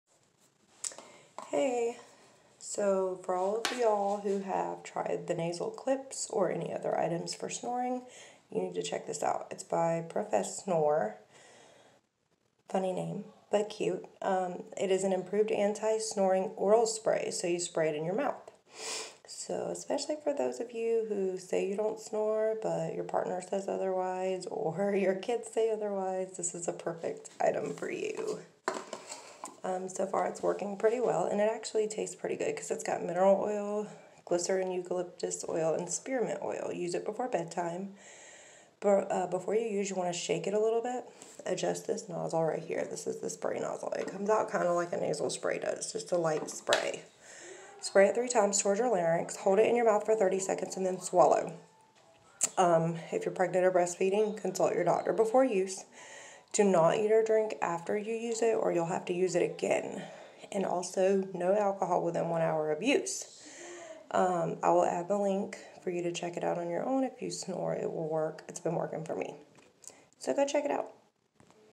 Professnore!! Oral snoring spray!!! Who knew?
I have never heard of anti snoring ORAL spray before now! Thia is great! I absolutely hate trying to sleep with clips or nasal widening items! If those items drive you nuts too, this spray is for you! It tastes good too! Its cost friendly, and found on ebay! Check it out here!